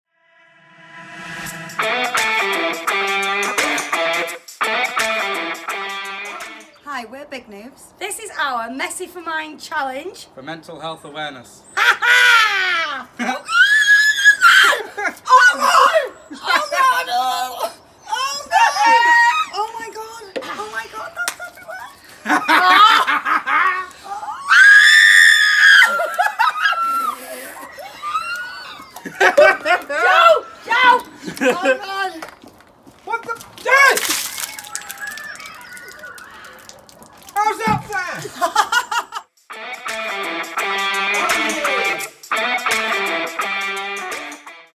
BiigNoobs does Messy4Mind
Recently we were asked by Messy4Mind if we would get messy in aid of mental health awareness. As something that is very close to our hearts we, of course, said yes!

So please enjoy some messy fun with the BiigNoobs and why not get involved with your own messy challenge, who doesn't love a little gunge!